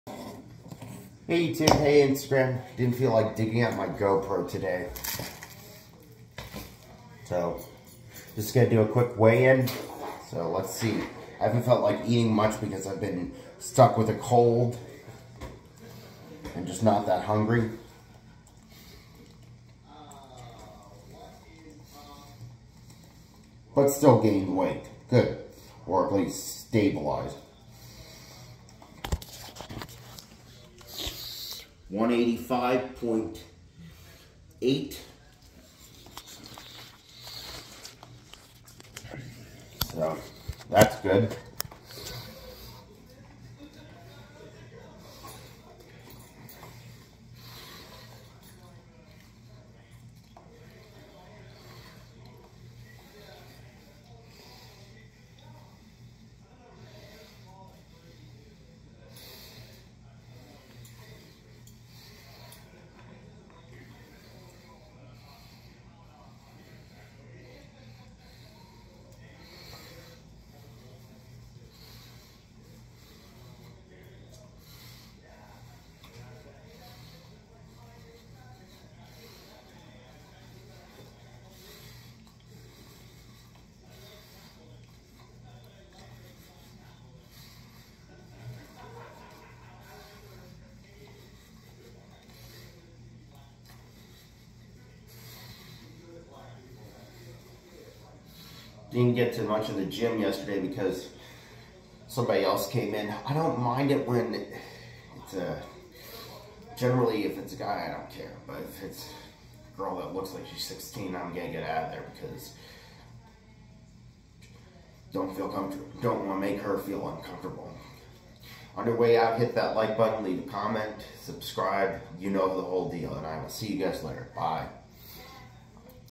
weigh-in and belted belly play
I haven't had much of an appetite due to being kind of sick. so I think my weights are a little down, but it's going okay. I hope everybody enjoys this.